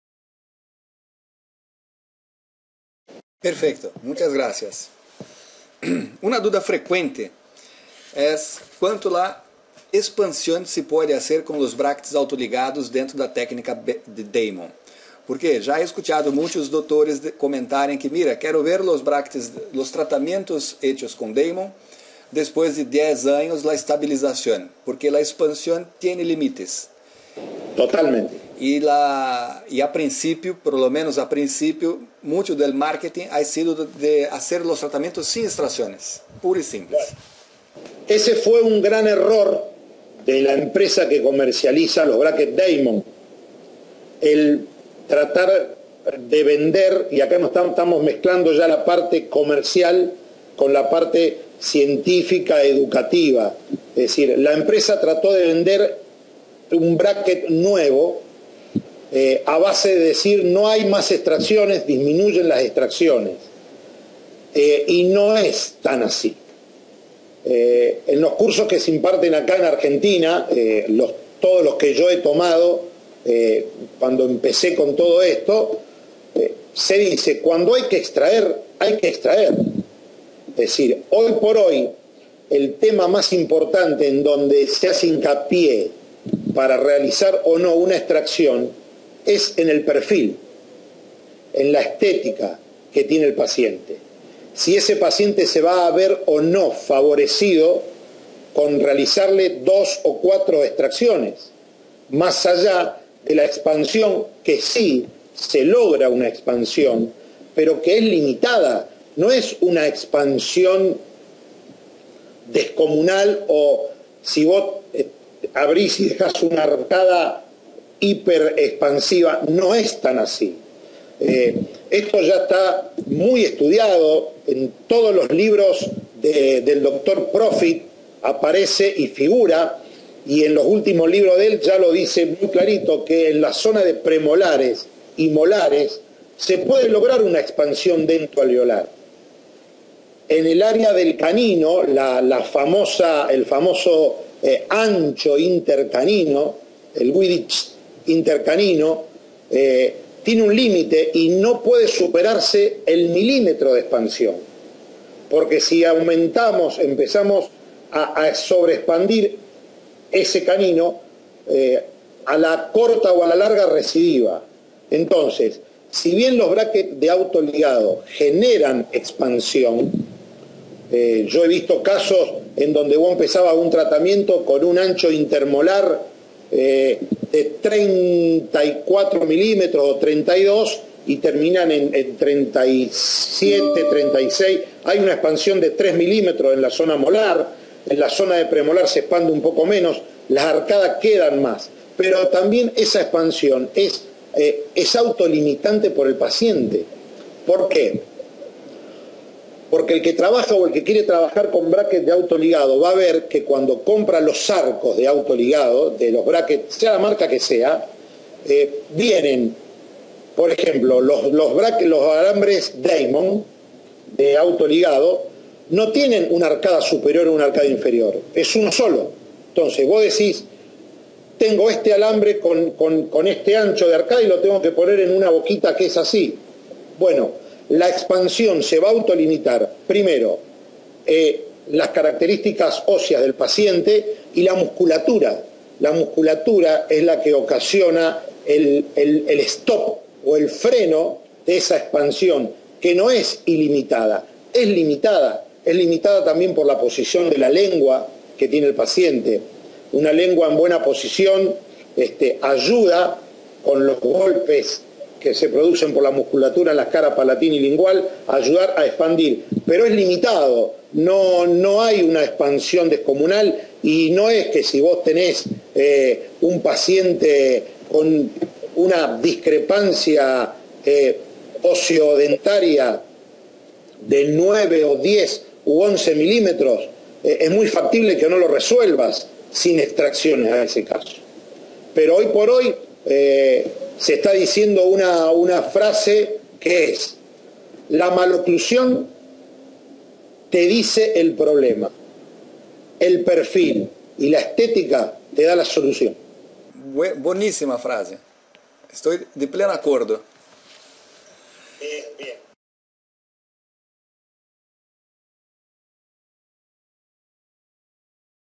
Edigardo Borraccetti 4 expansion.mp4
Odontolunes, Dr. Edgardo Borraccetti comenta la expansion con braquetes autoligados pasivos.